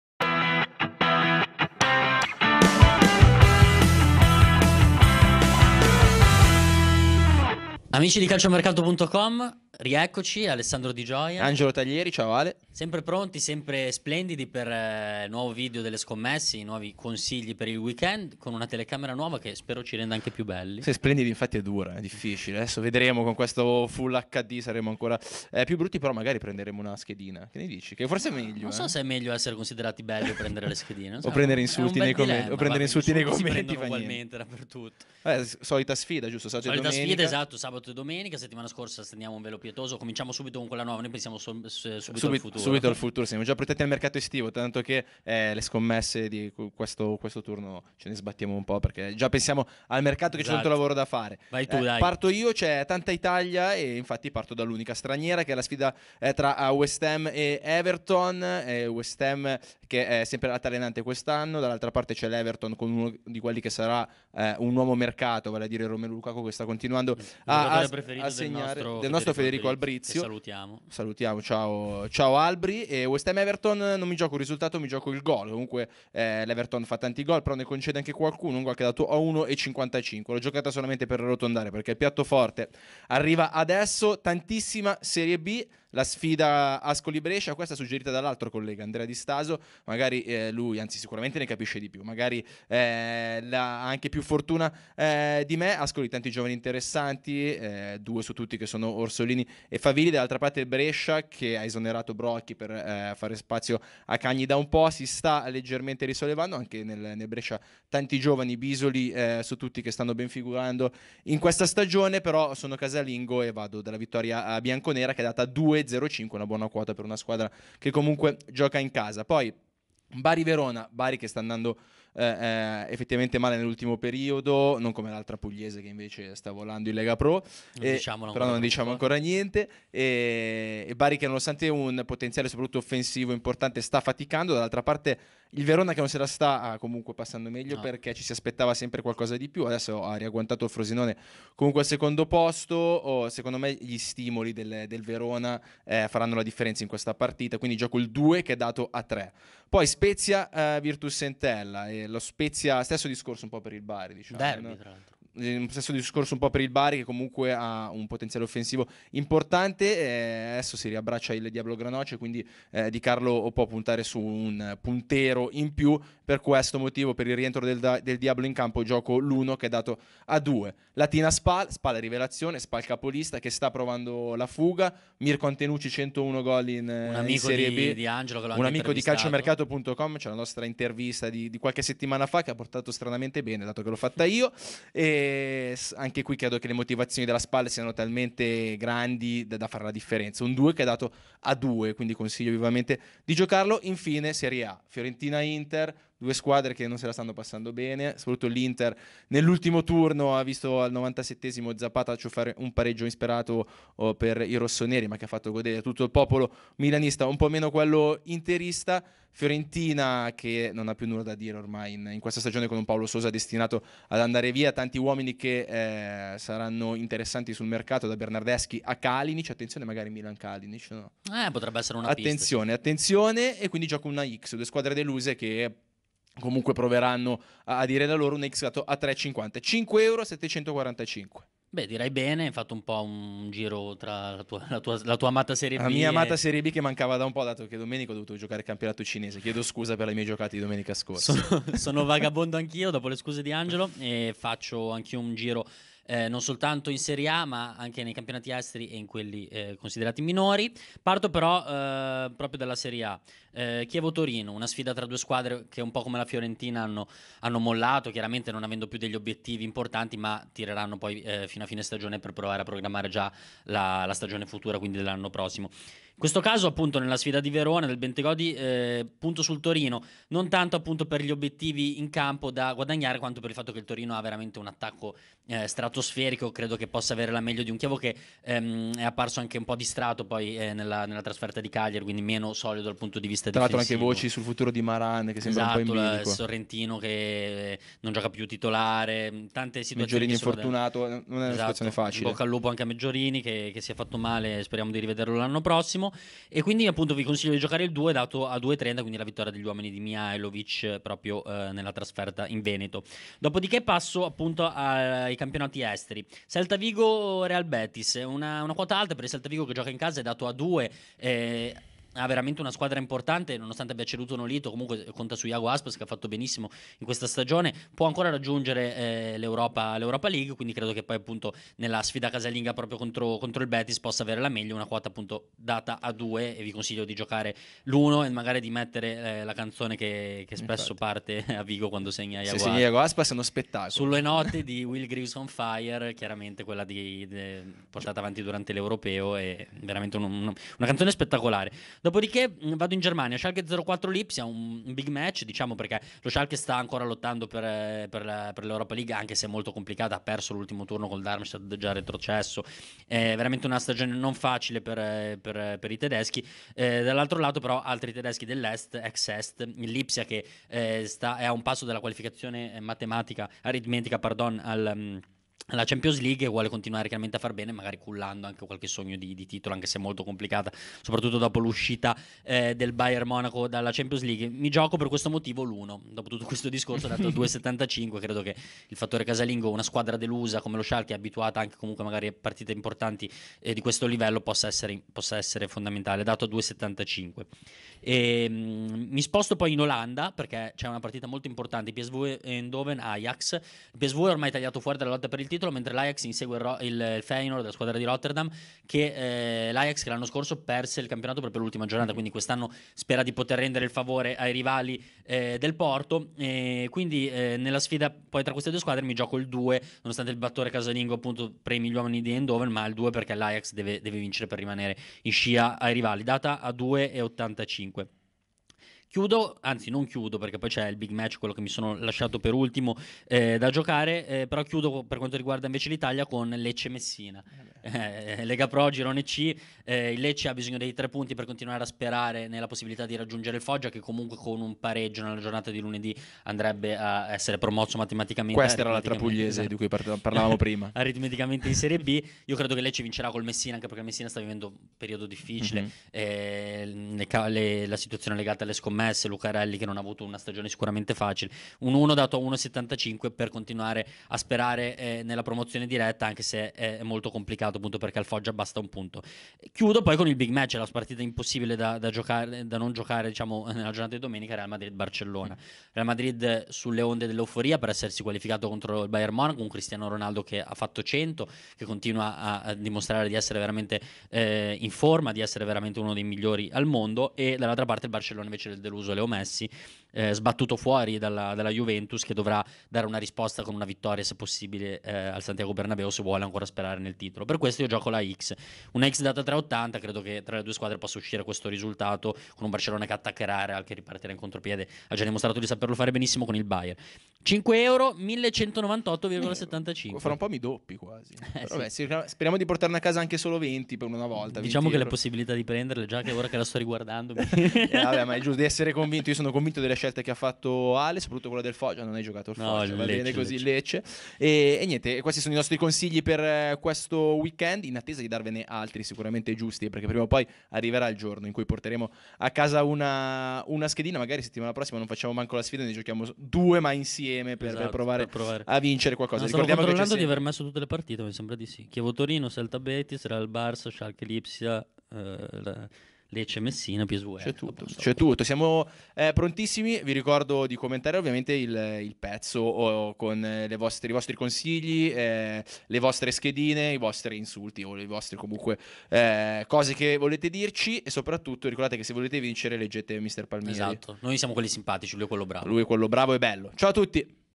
Schedina di CM: ecco su cosa scommettere il 22 e 23 aprile
I consigli su cosa scommettere nel weekend internazionale.